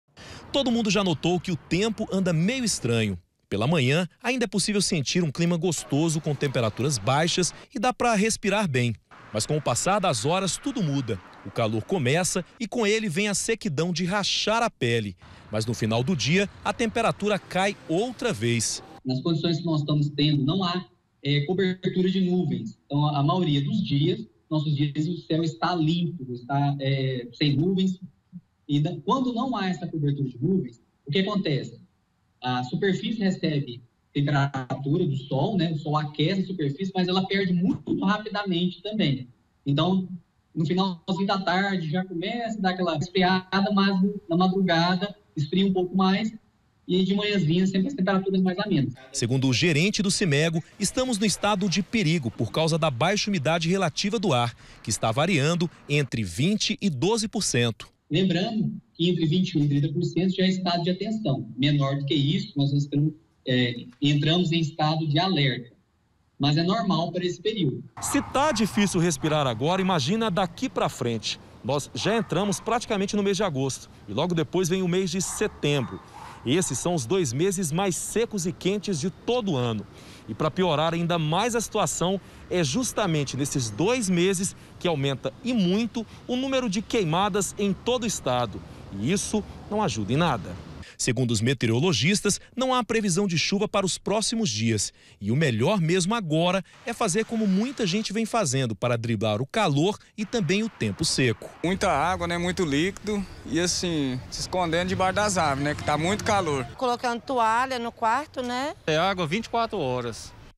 TEMPO SECO: BAIXA UMIDADE DO AR DEIXA O ESTADO EM ALERTA
ALÉM DO CORONAVÍRUS, O GOIANO TEM MAIS UM PROBLEMA PRA SE PREOCUPAR A PARTIR DE AGORA: É O TEMPO SECO, QUE DEIXA A UMIDADE DO AR CADA VEZ MAIS BAIXA. DURANTE A TARDE, A UMIDADE DO AR EM GOIÁS TEM FICADO ABAIXO DE 20% ISSO SIGNIFICA QUE ESTAMOS EM ESTADO DE ALERTA. ESPECIALISTAS RECOMENDAM TOMAR MUITA ÁGUA PRA EVITAR PROBLEMAS DE SAÚDE. E SEGUNDO A METEREOLOGIA, NÃO ADIANTA PROCURAR NUVEM NO CÉU: NÃO TEM NENHUMA PREVISÃO DE CHUVA.